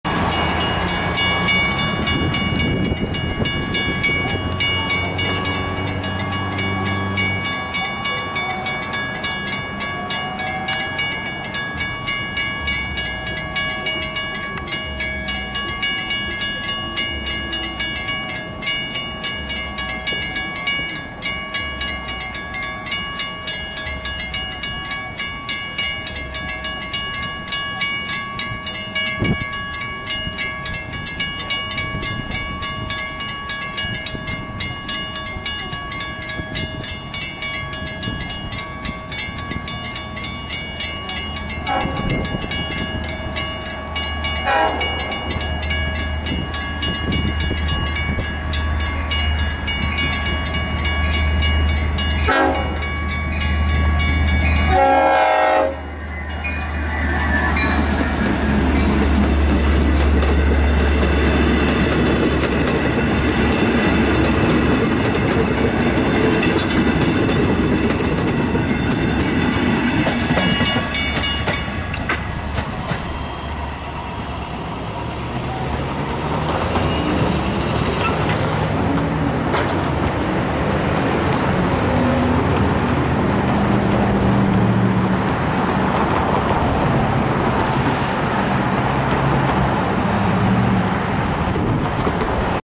LIRR 2706
2706 destined for Montauk stops at Mastic Shirley with DE30AC #413 on point and C3 #5003 trailing.